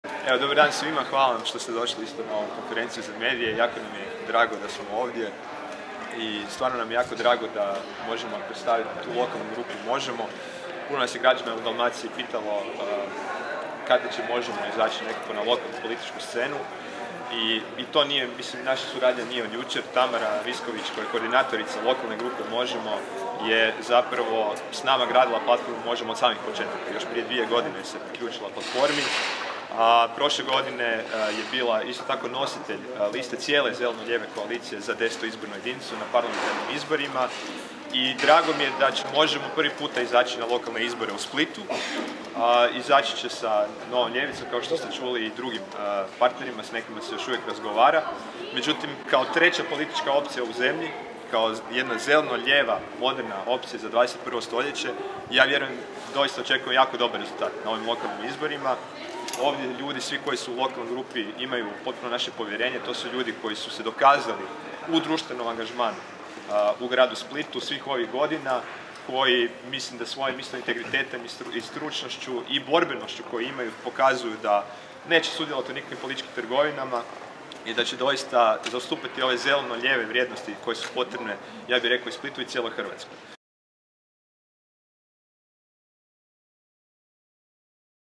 U Splitu najavljeno osnivanje lokalne grupe političke platforme Možemo!
Konferencija za medije na kojoj je najavljeno osnivanje lokalne grupe političke platforme Možemo! Na konferenciji za medije govorili su Tamara Visković i Ivica Profaca u ime inicijative grupe Možemo! Split, te Sandra Benčić i Tomislav Tomašević u ime platforme Možemo!